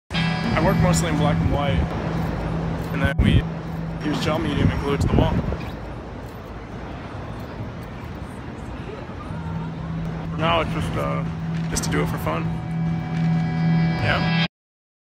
Artist, Skateboard Dude, Muralist—Jourdon Gullett_Credstyle Films
Artists helping an artist post a street-style mural— created by Jourdon Gullett at @SohoHouse Chicago on Green street.